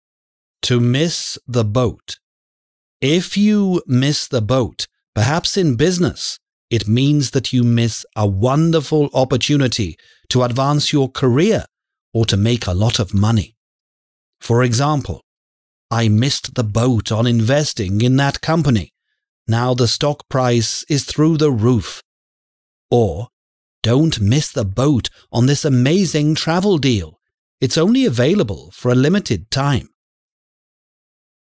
Useful Idioms 114: Miss the boat
Meaning:
This means to miss a wonderful opportunity, likely a business opportunity that could have made a lot of money.

Examples:
"I missed the boat on investing in that company. Now the stock price is through the roof."
"Don't miss the boat on this amazing travel deal. It's only available for a limited time."